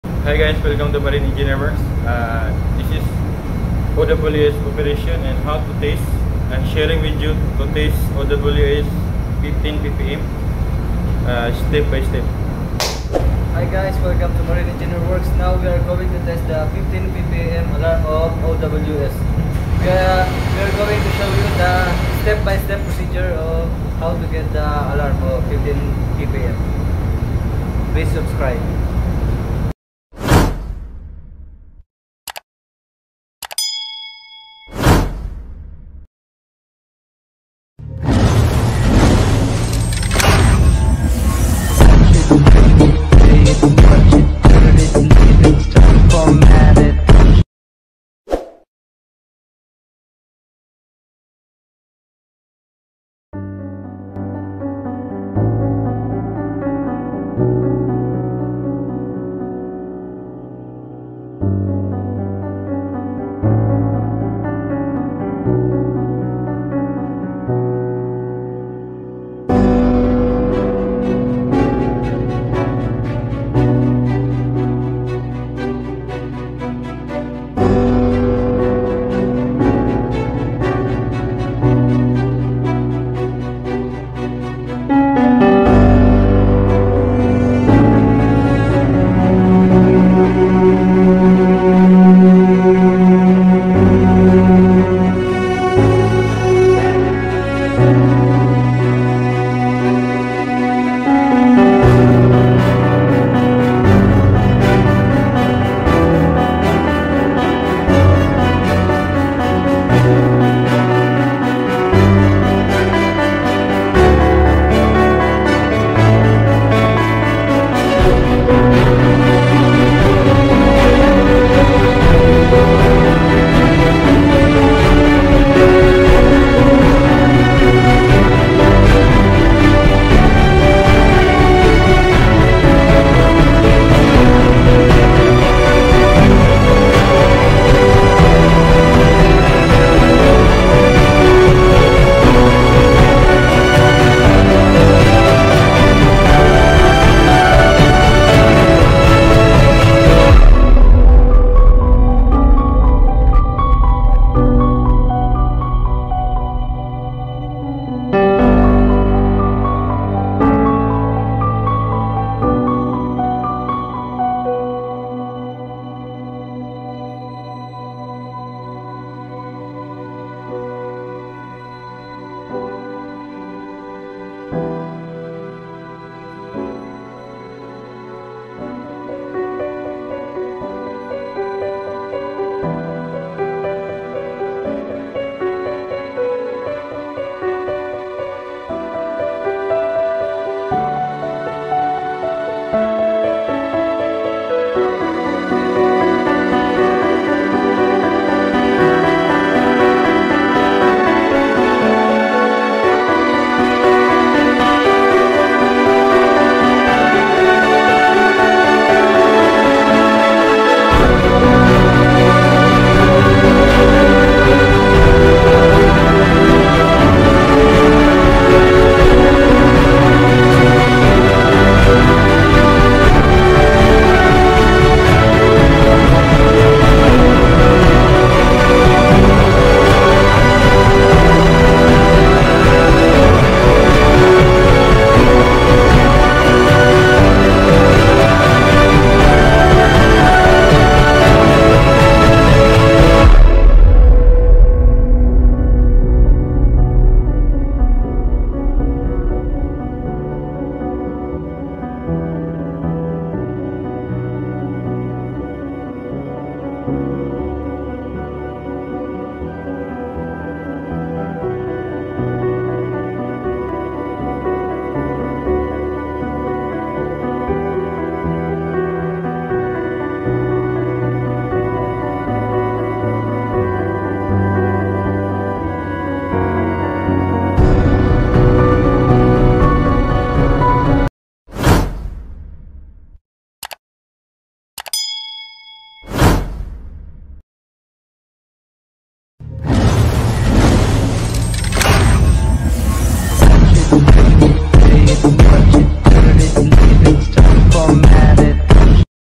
oily bilge water separator tried out step by step with out fail.
how OWS works 15ppm tried out...
 this kind of vlog learn you from small thing to bigs things..
thank you to all who watch my video.
keep safe and have and have a nce day..